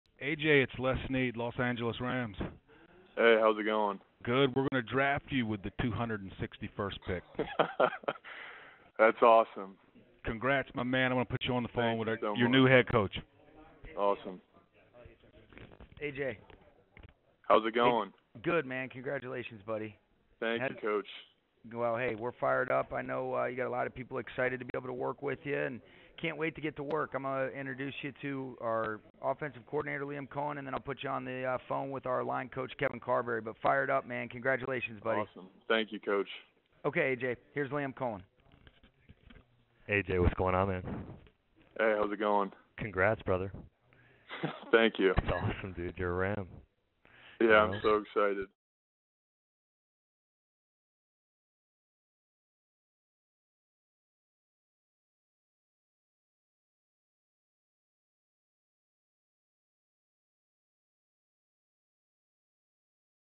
"Can't Wait To Get To Work!" New Rams OT AJ Arcuri Gets The Call From Liam Coen & Sean McVay
Listen in as Michigan State offensive tackle AJ Arcuri gets The Call from Los Angeles Rams general manager Les Snead, head coach Sean McVay & offensive coordinator Liam Coen. He was selected in the 7th Round of the 2022 NFL Draft.